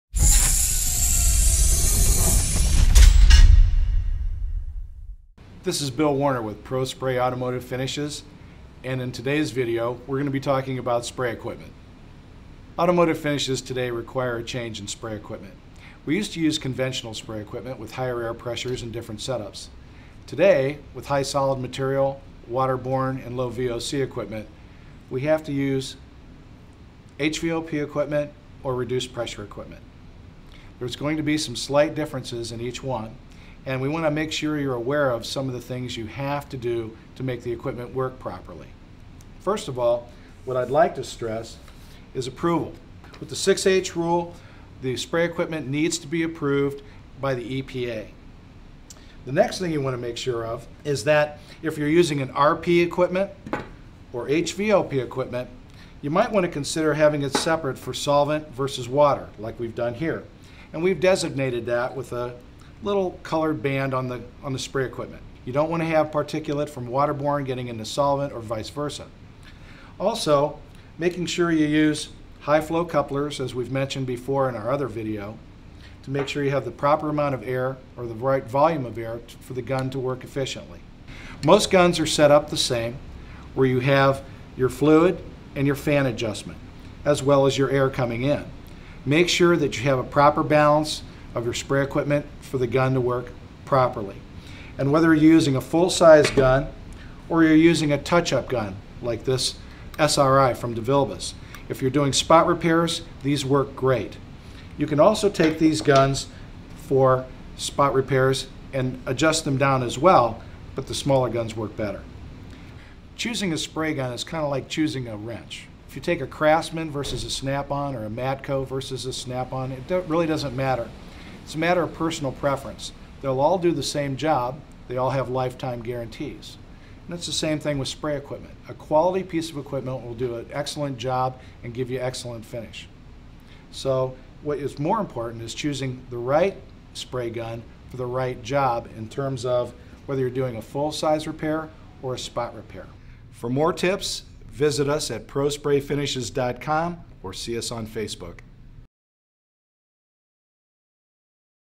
Spray Gun Basics 1: An Overview of Automotive Spray Equipment
Today's high-solids coatings, waterborne basecoats and low VOC products require HVLP or reduced pressure spray guns.  These guns vary from the equipment of the past.  Navigating the options can be confusing.  Bill Warner, Pro-Spray technical trainer and instructor, simplifies the process by sharing his tips on choosing the right spray gun for the right repair.